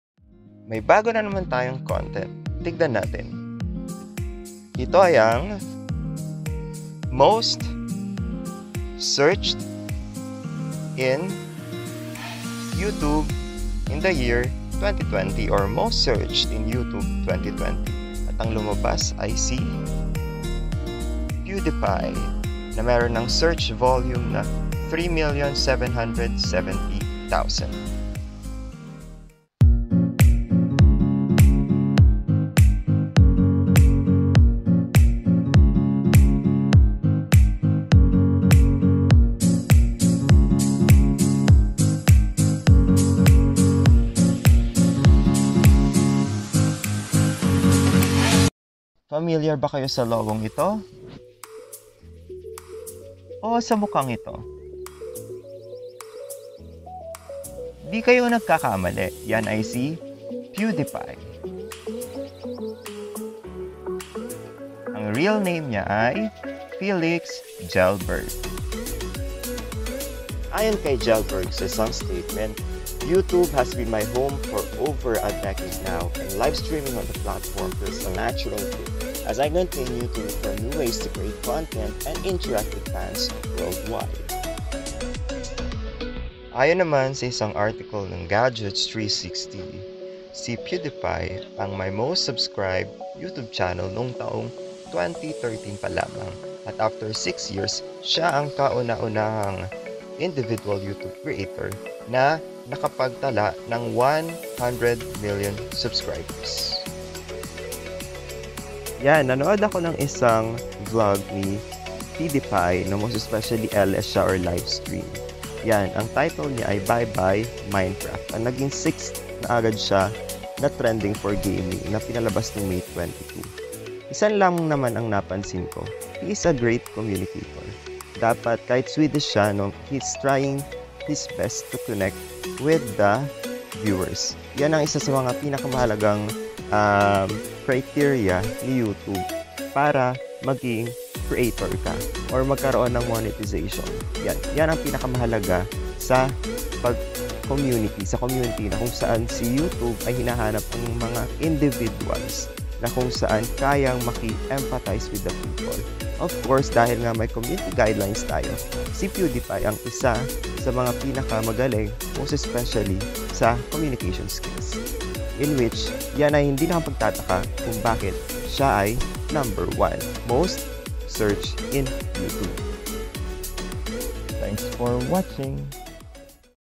Why did PewDiePie become the most searched in YouTube for the year 2020? (A Review)
Bakit nga ba si PewDiePie ang most searched in YouTube? Panoorin ang short review na ito. Thanks a lot sa lahat ng sumusuporta ng Jas Per Day